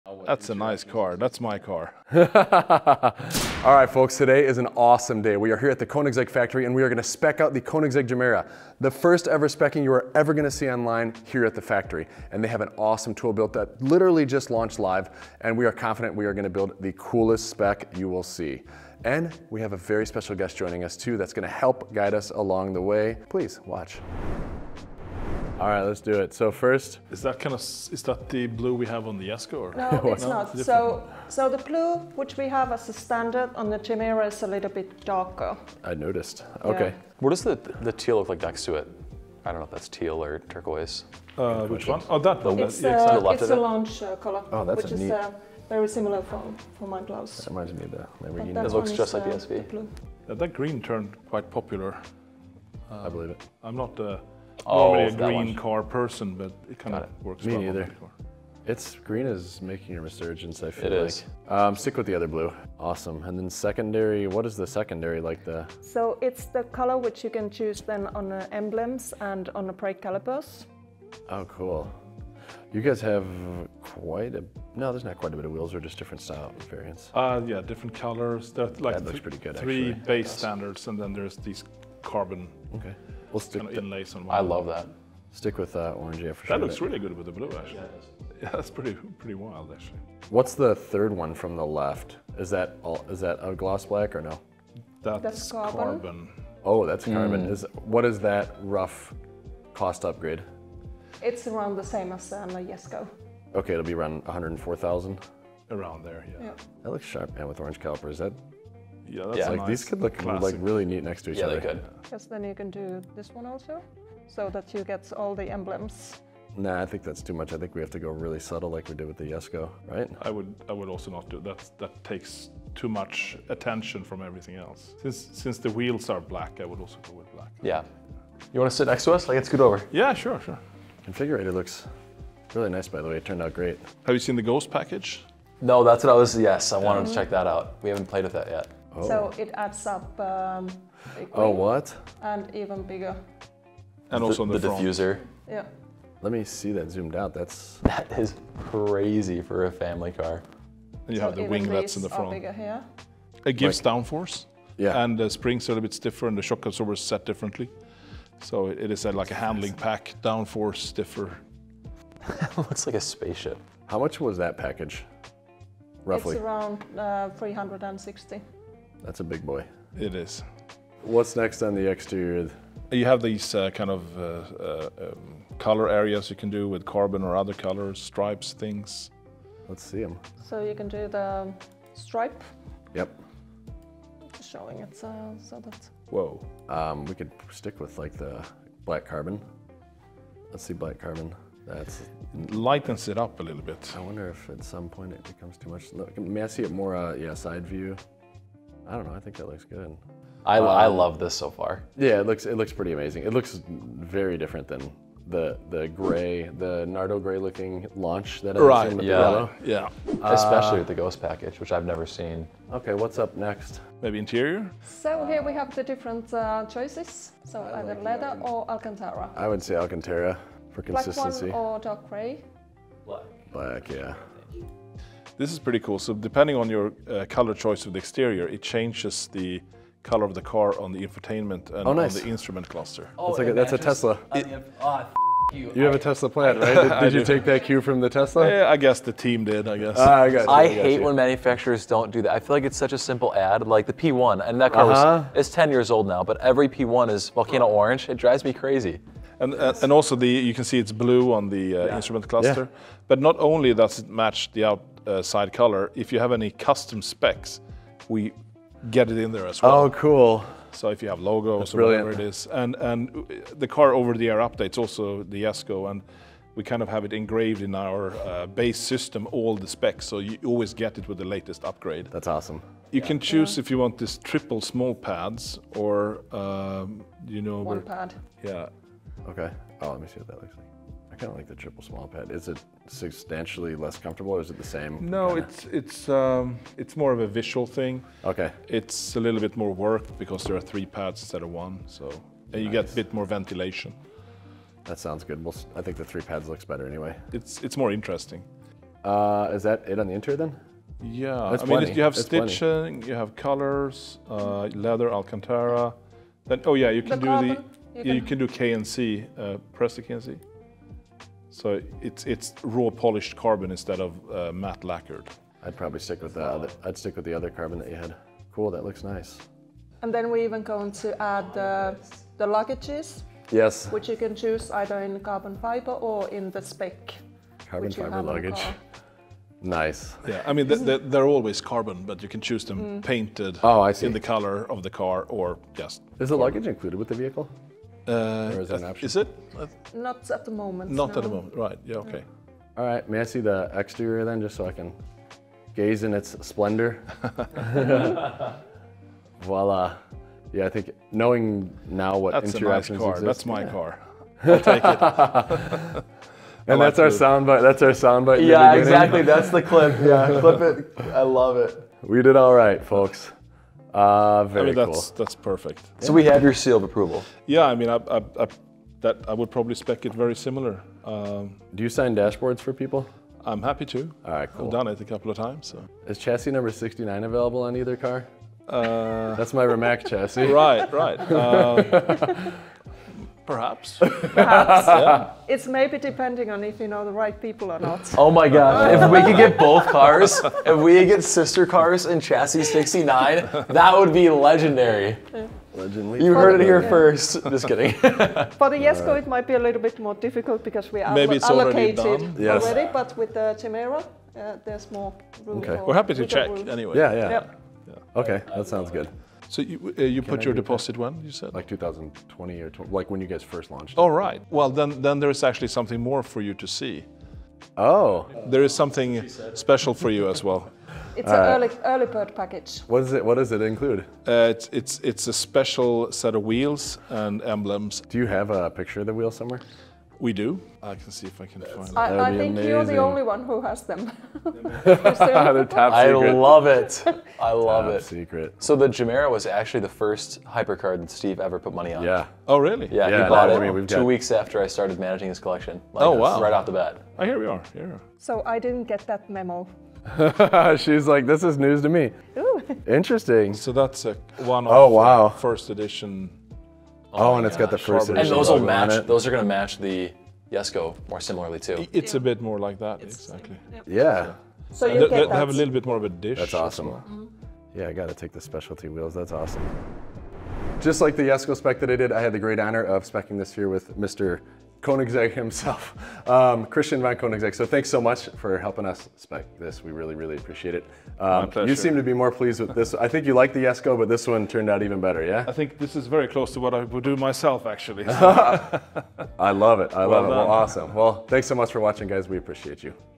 My Koenigsegg Gemera- World's First Spec Session!
World's First Koenigsegg Gemera Speccing at the Factory with Christian Von Koenigsegg!

New vlogs every Wednesday.

Steve Hamilton: @Steve.Hamilton1 (Owner)
Tommy Gauthier: @TommyG.THC (Collection Manager)
Natalia Kaczor: @Nat.THC (Brand Director)
Aidan Faber: @Aidan.THC (Videographer)